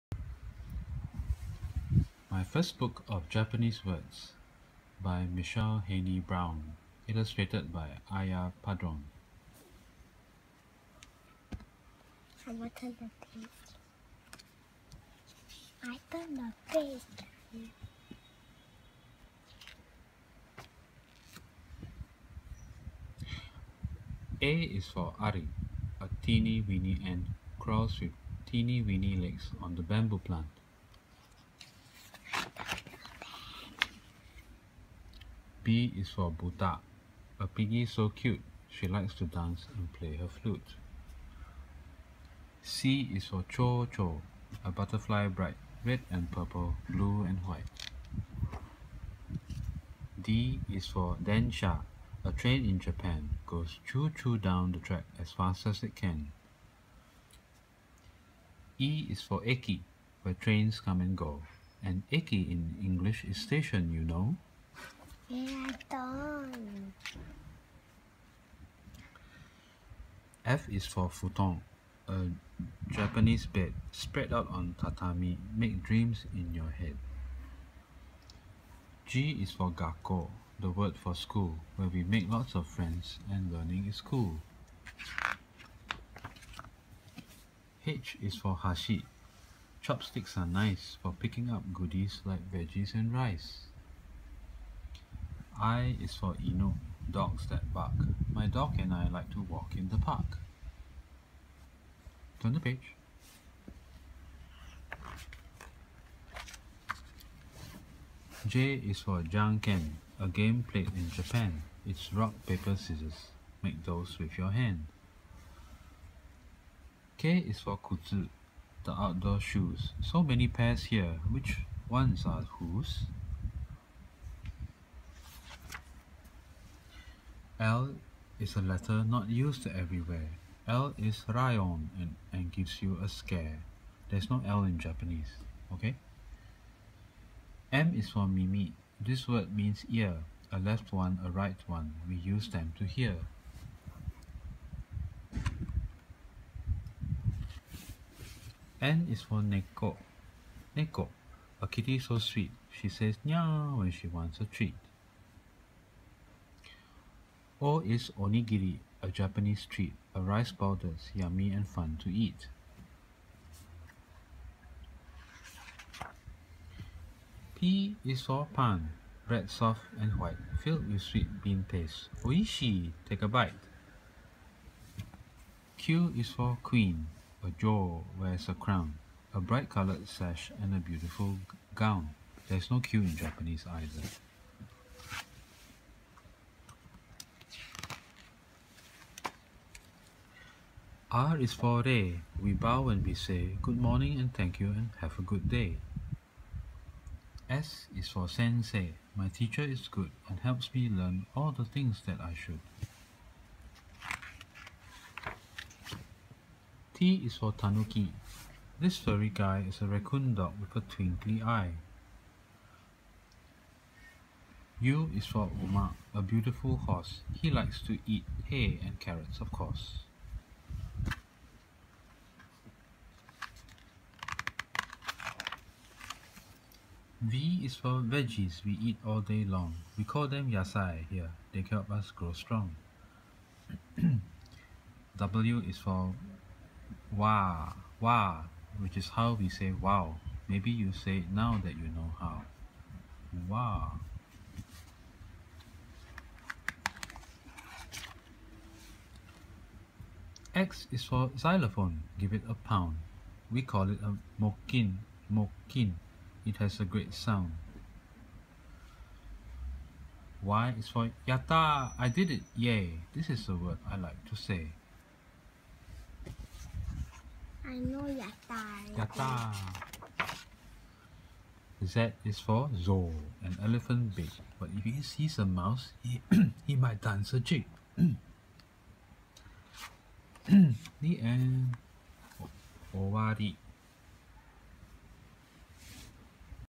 Nobody's Books #12: My First Book of Japanese Words by Michelle Haney Brown
My little girl wanted to learn some Japanese as she has a half-Japanese classmate in her violin class, so my wife borrowed this for her.